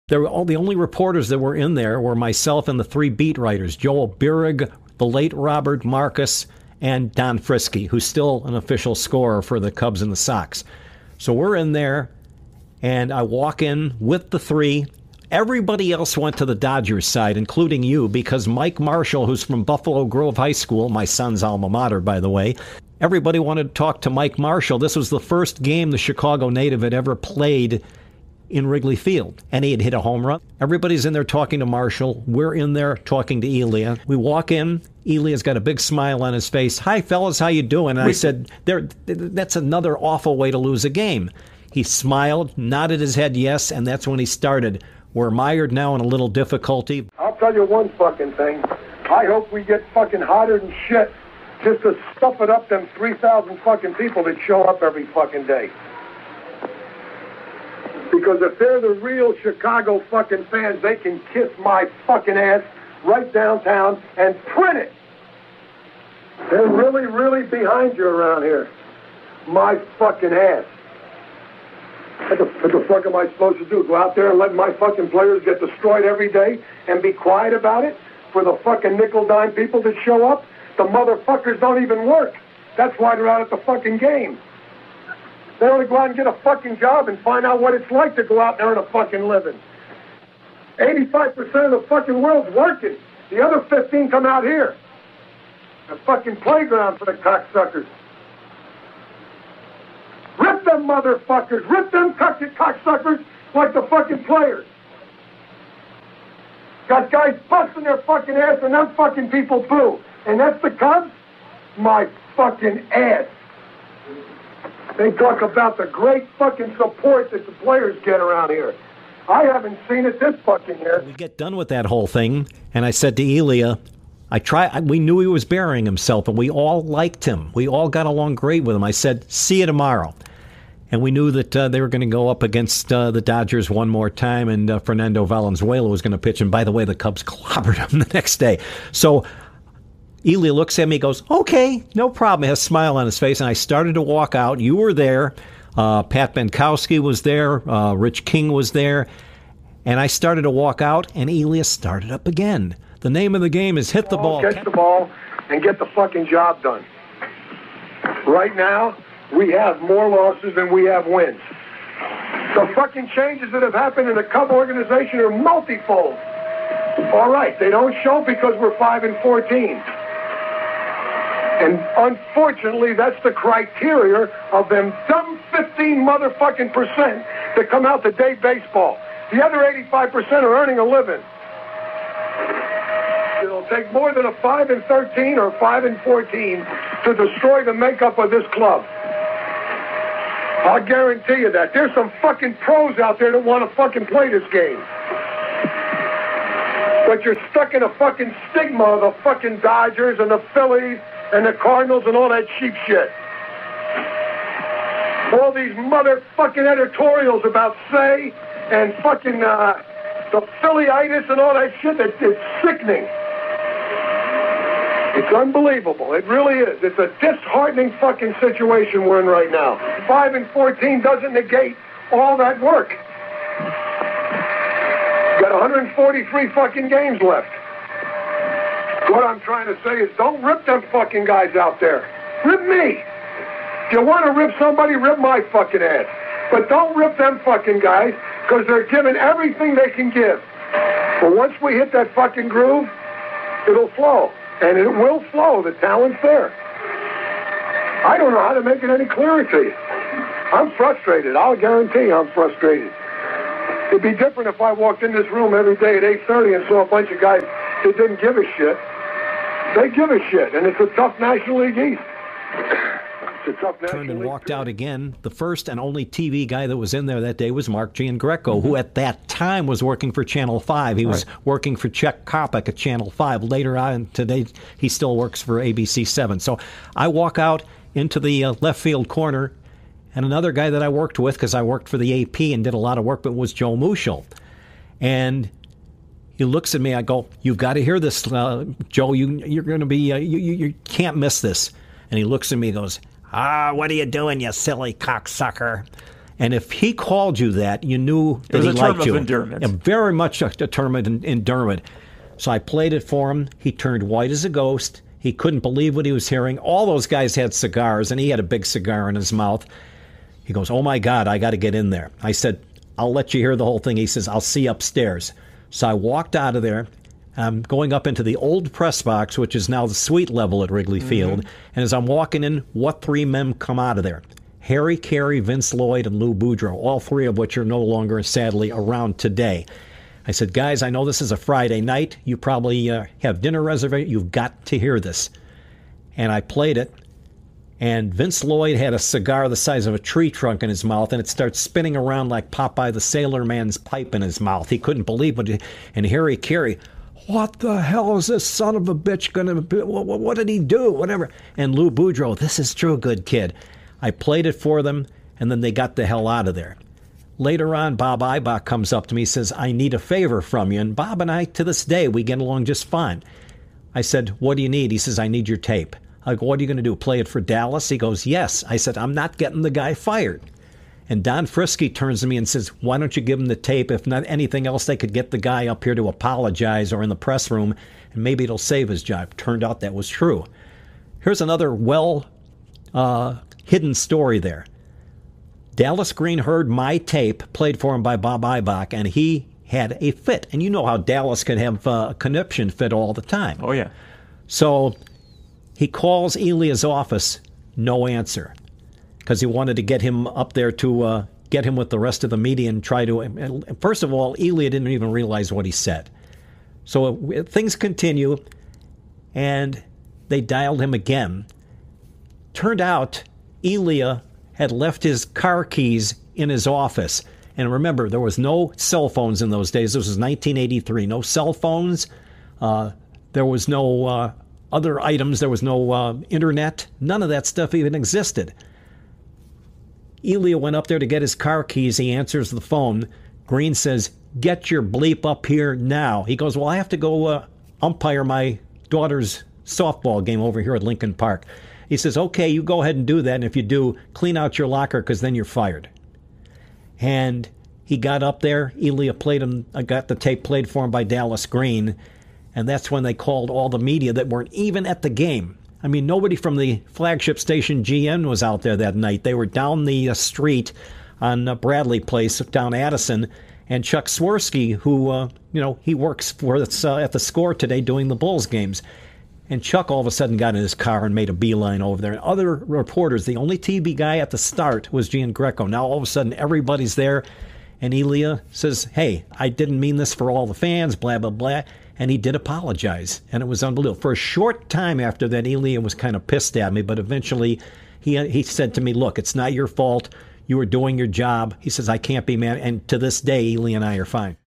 Grobber Recalls Elia Rant
The late Les Grobstein recalls the day he recorded the infamous Lee Elia rant.
WARNING: OBSCENE LANGUAGE